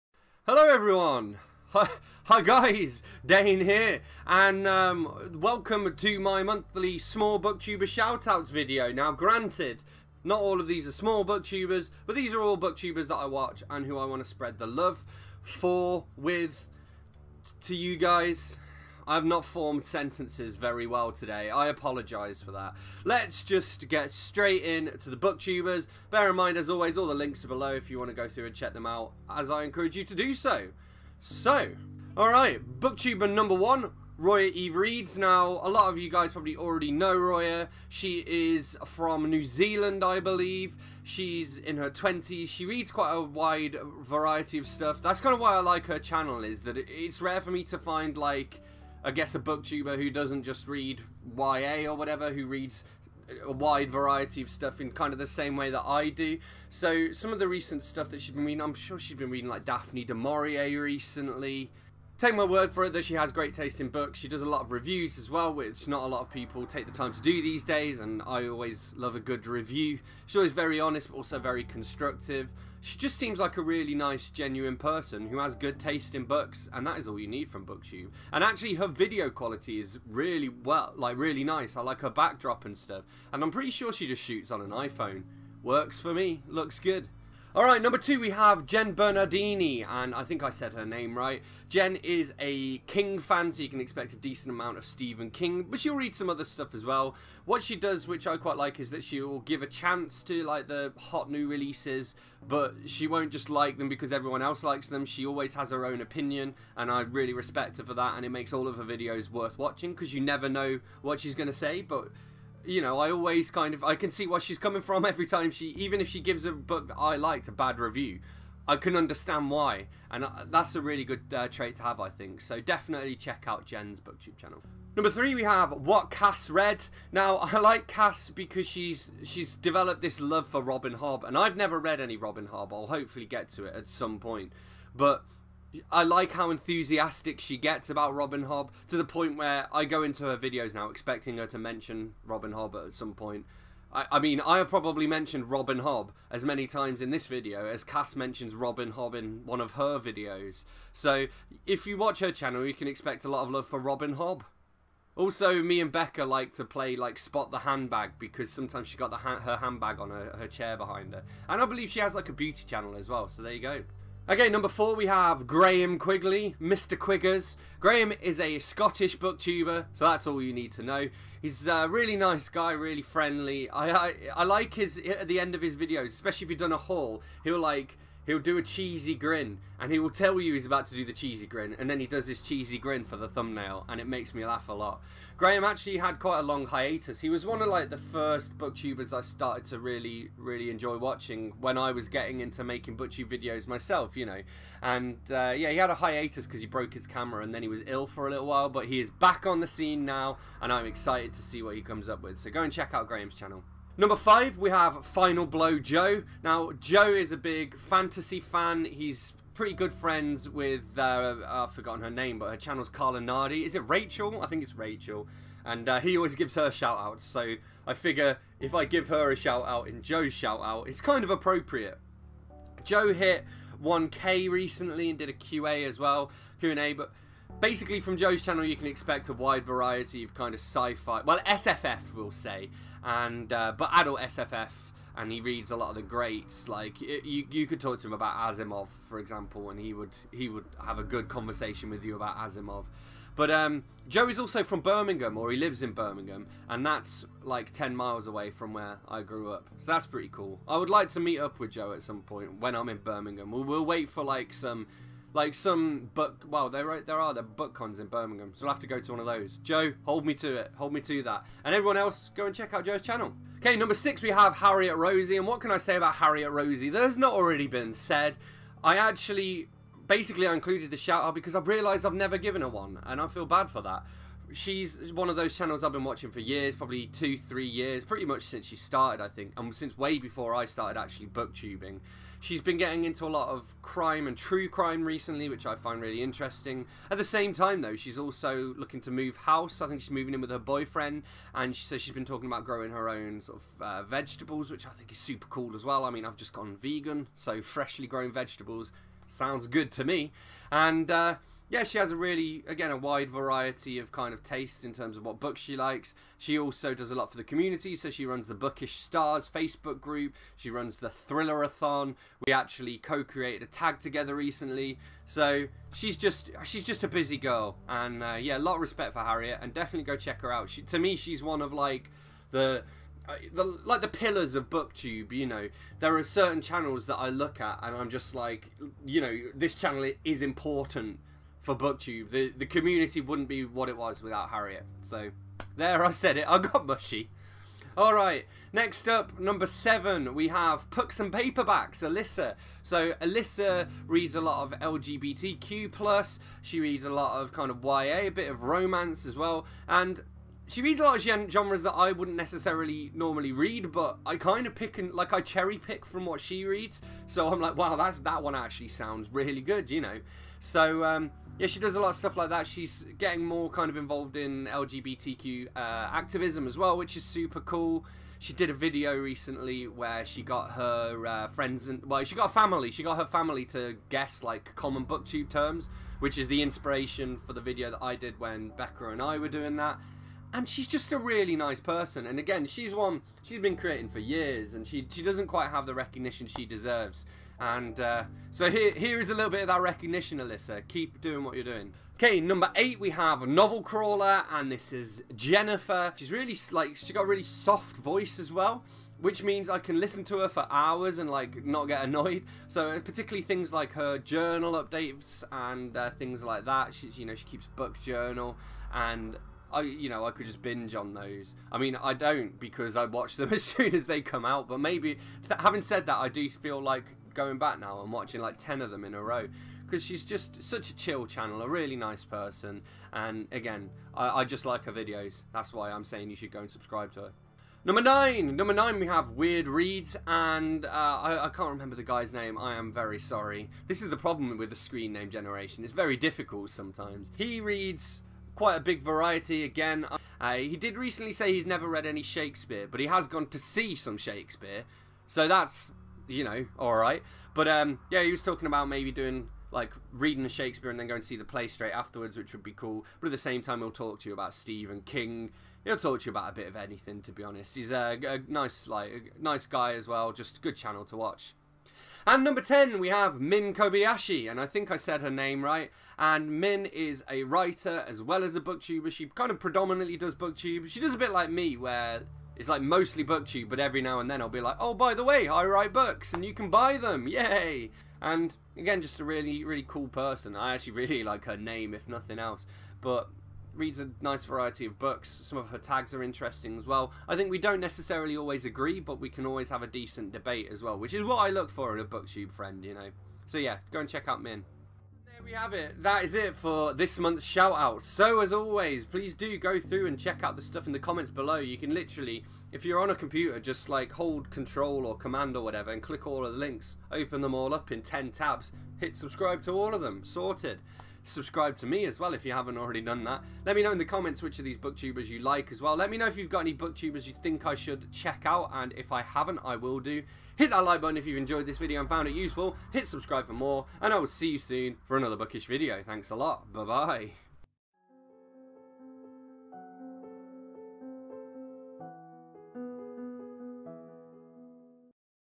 Small BookTuber Sunday #5: Ten More Channels to Watch!
More people you should watch. I was going to post this next Sunday, but my wrapup isn't ready yet and so what the hell? :D